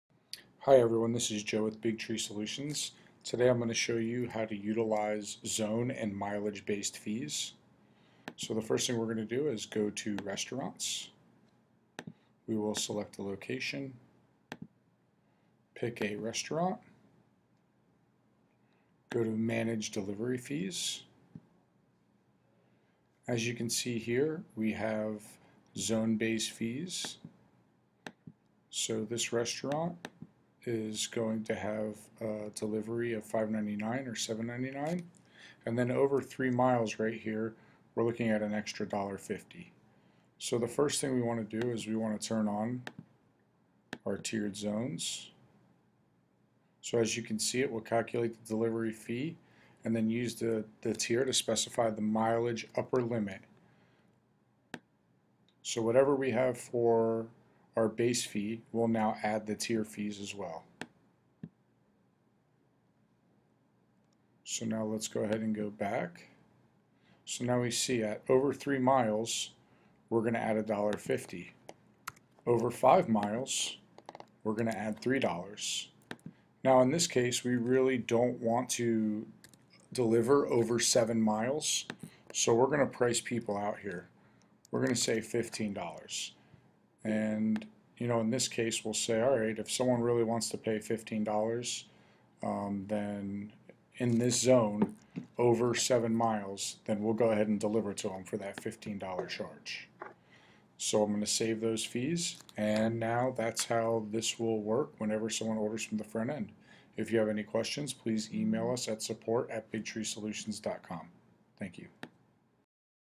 Software Setup - Tiered Milage Based Fees
This video shows you how to utilize tiered milage based fees along with zone fees.